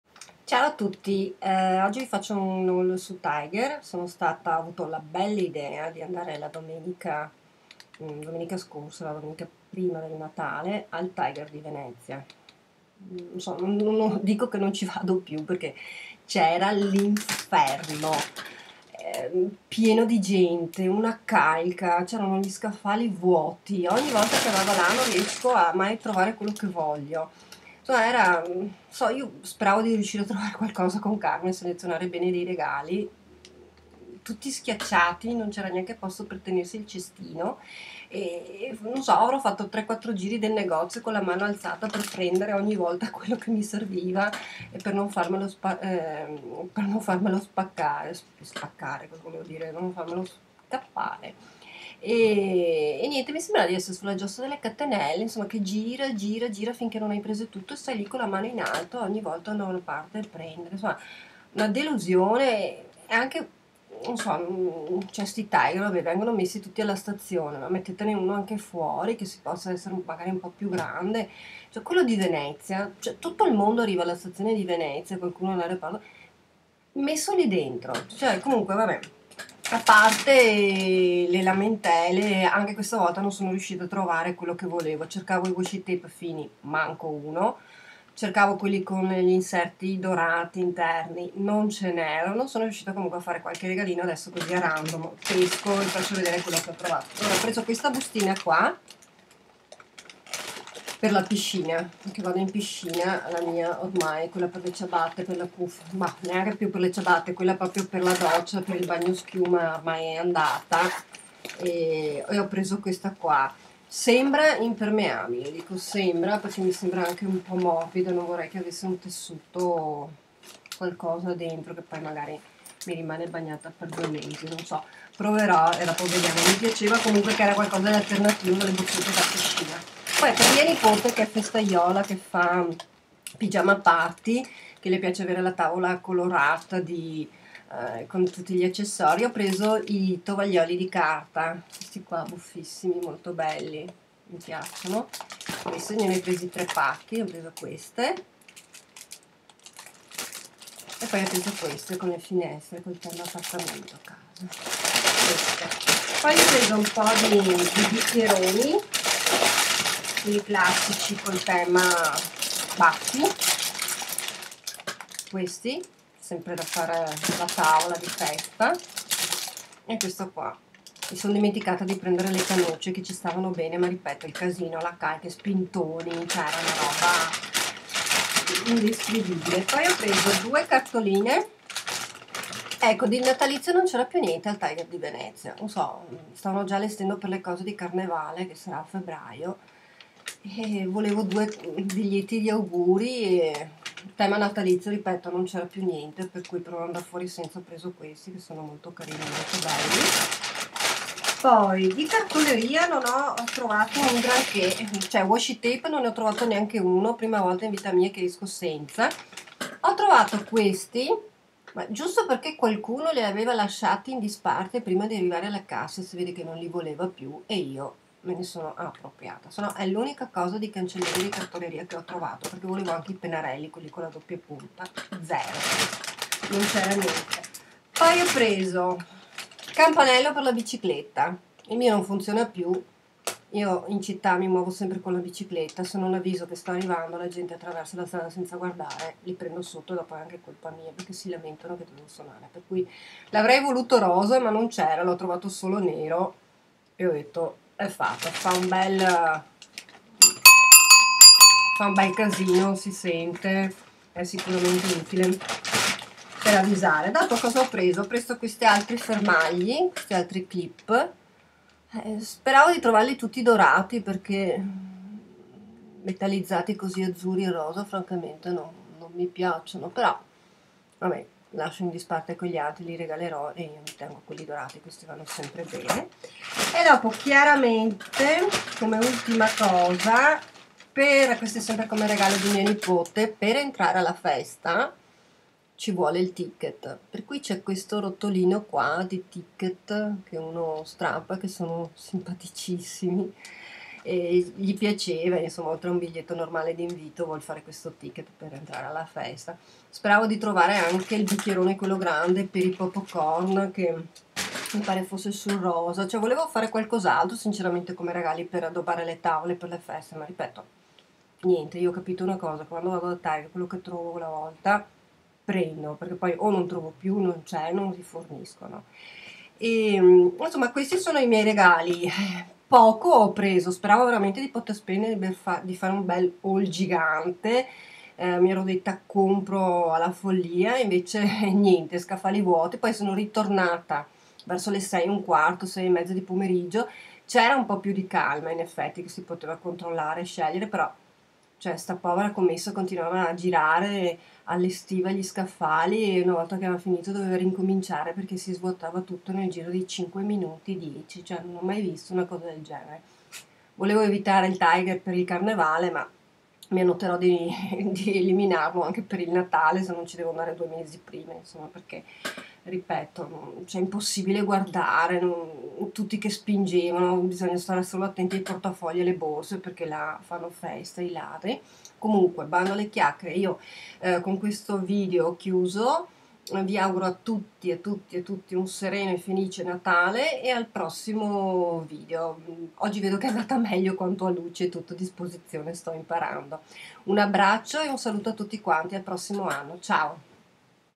TIGER DI VENEZIA... NON ENTRATE !!!!!
Haul Tiger di Venezia a ridosso del Natale..un disastro, comprato pochissimo, Vi faccio vedere comunque qualcosina
Il tuo supporto e la tua fiducia per me sono molto importanti.